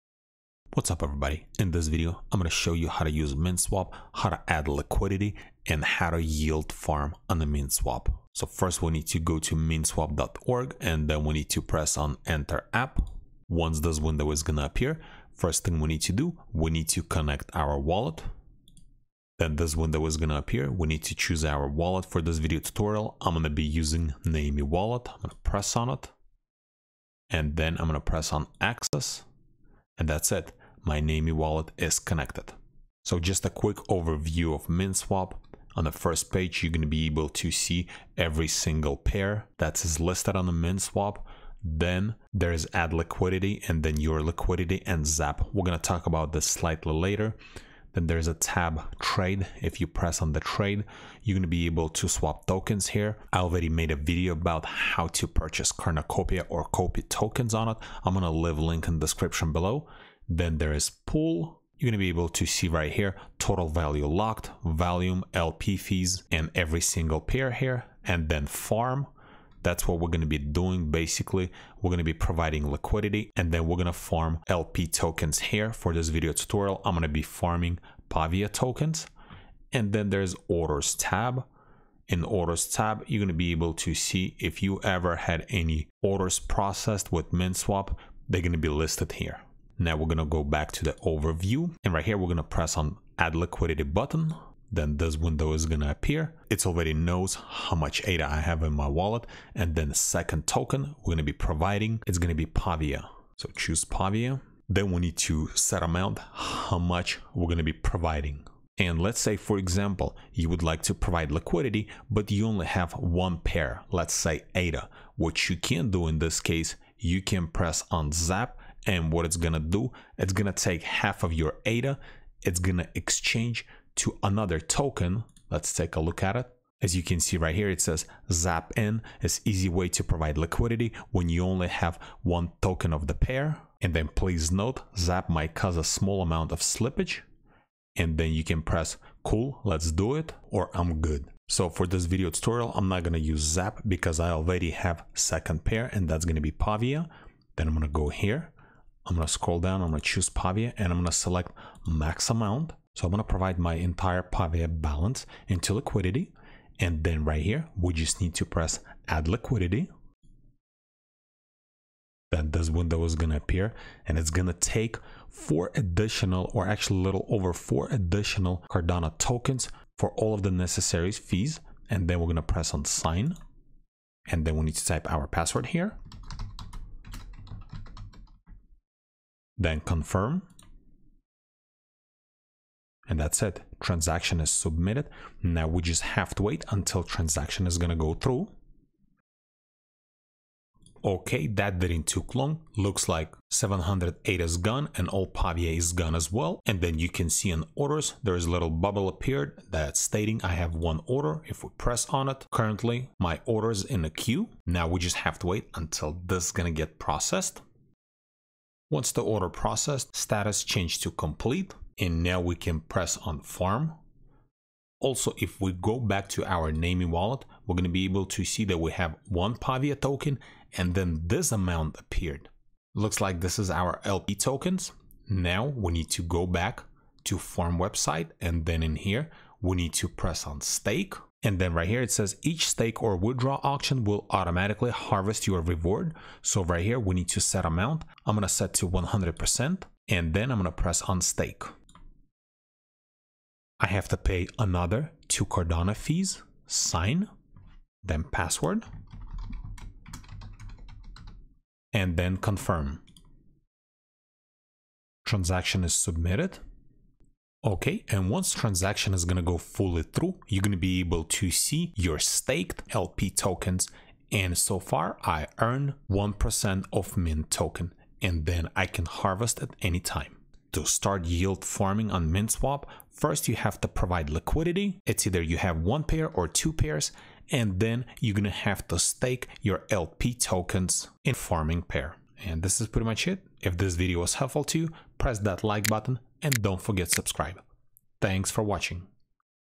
How to Provide Liquidity and Yield Farm on MinSwap
In this video I will show you step by step and how to provide liquidity and start Yield Farm on Cardano MinSwap DEX.

PERSONAL GEAR
4k Asus Monitor
Video Camera Tripod